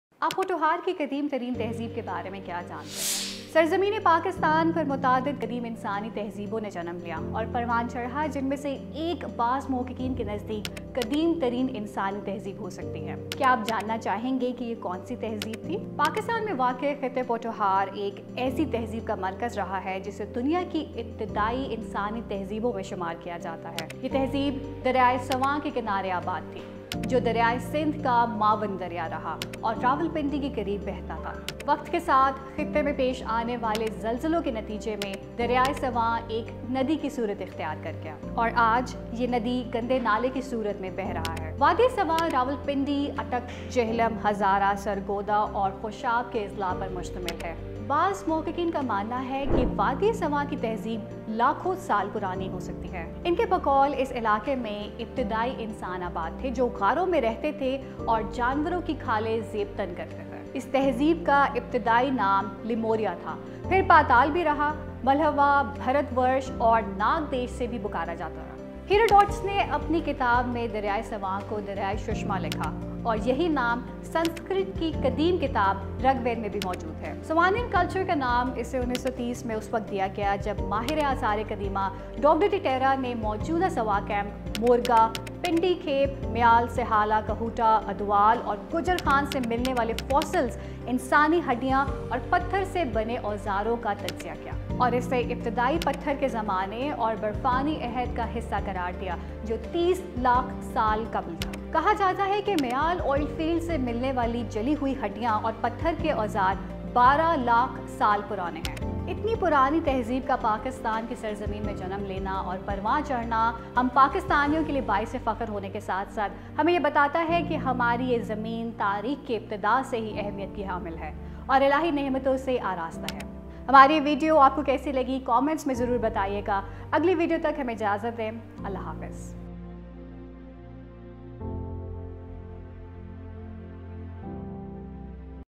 Ancient Civilizations in Pothohar
The history of Pothohar in this region dates back to ancient civilizations. Evidence has been found that the Soan and Indus Valley civilizations also occupied this area.